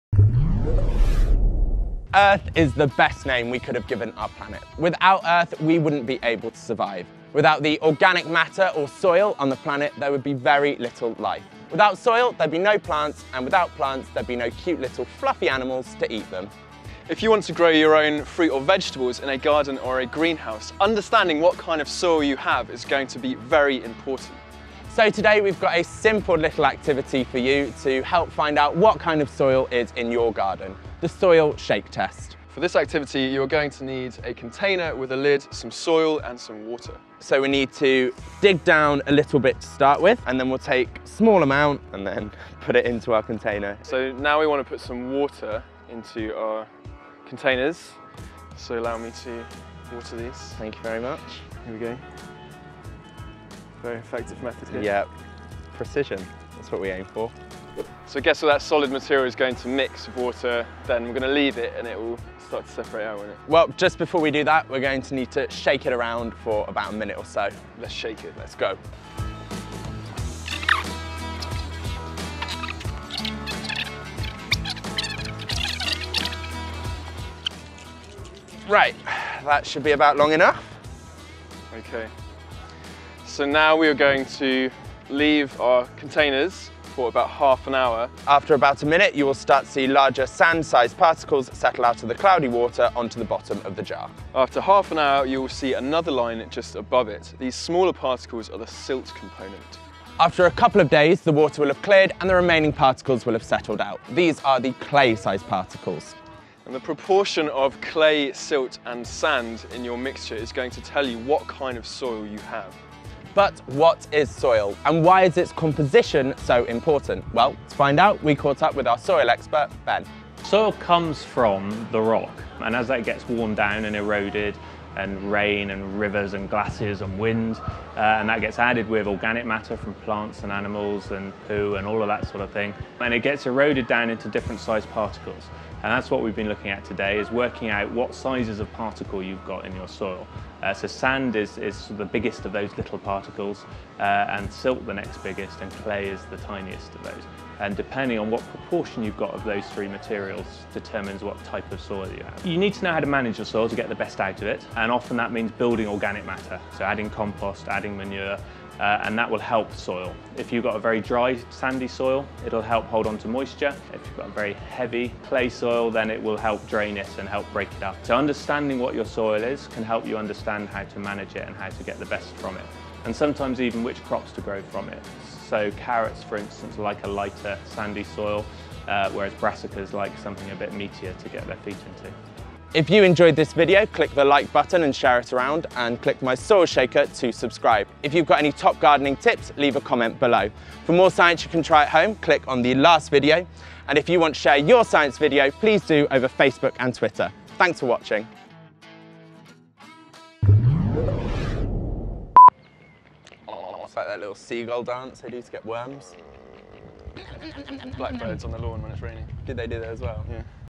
How to test the soil in your garden | Do Try This At Home | We The Curious
Soil is essential to life as we know it on planet Earth. This week, Robbie and Joel show you a simple activity to try at home to investigate what kind of soil you might have in your garden.

This video was presented by: Robbie Kornitschky and Joel Sanderson, Live Science Team.
Produced by: Ross Exton, Live Science Video Producer

Special thanks to the Soil Association and Ben Raskin.
For more information on soil and some top gardening tips, check out